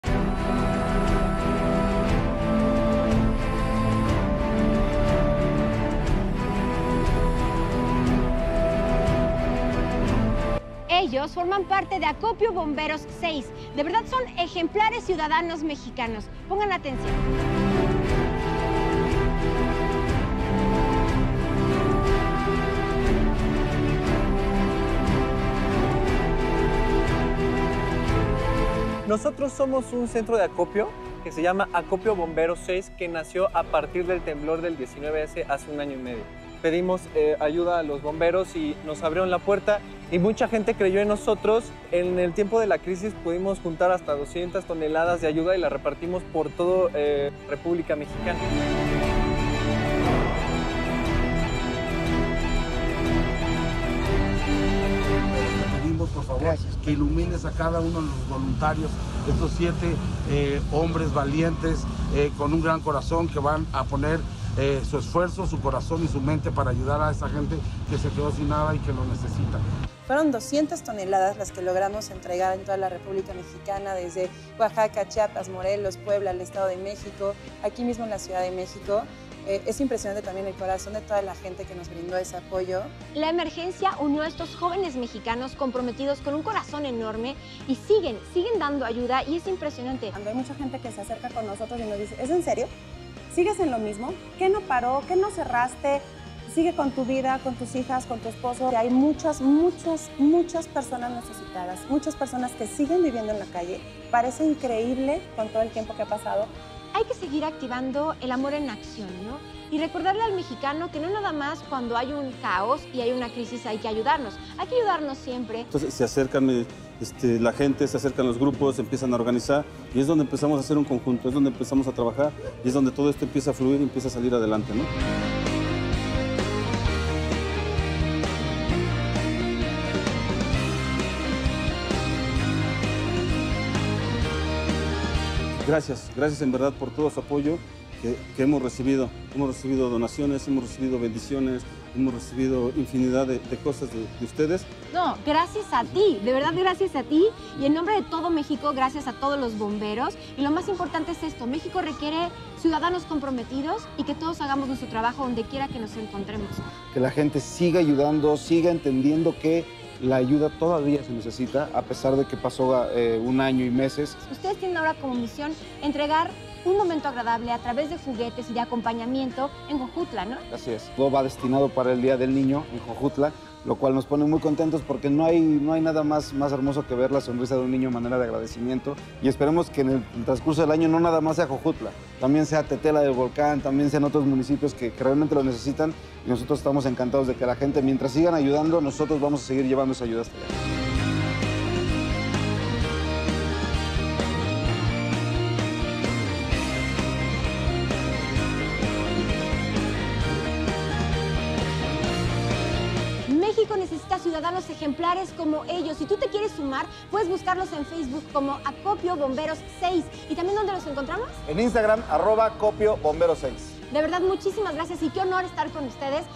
ACOPIO 6
La emergencia del 19 de septiembre del 2017 con el temblor, generó una unión de todos los mexicanos, y surgió un grupo de civiles que junto con los bomberos de Tlalpan ayudaron a mucha gente. hoy siguen apoyando a las comunidades damnificadas  y tú también puedes sumarte, Acopio 6 siempre hace actividades y ahora irán a entregar juguetes a Jojutla el 28 de abril 2019 si quieres ayudarles aún puedes unirte